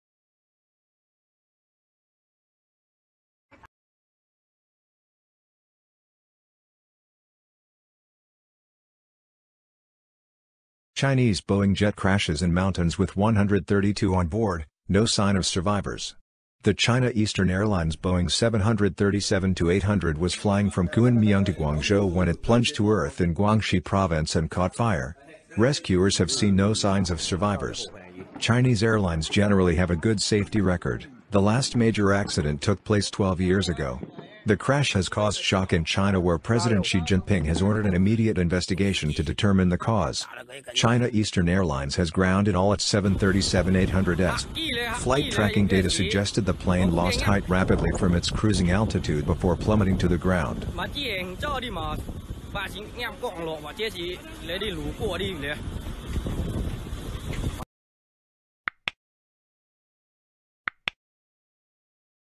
China Boeing 737 Tragically Crashed | carrying 132 | casualties unknown
●▬▬▬▬▬▬▬▬▬▬▬ஜ۩۞۩ஜ▬▬▬▬▬▬▬▬▬▬▬●
●▬▬▬▬▬▬▬▬▬▬▬ஜ۩۞۩ஜ▬▬▬▬▬▬▬▬▬▬▬●